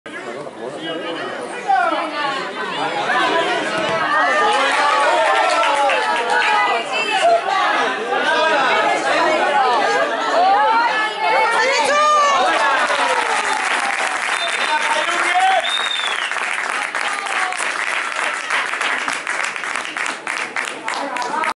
Gol Azkona (Eibar 1-6 Athletic B)
2ª División
Jornada 11 / 26-11-17
Autor: Noski Sky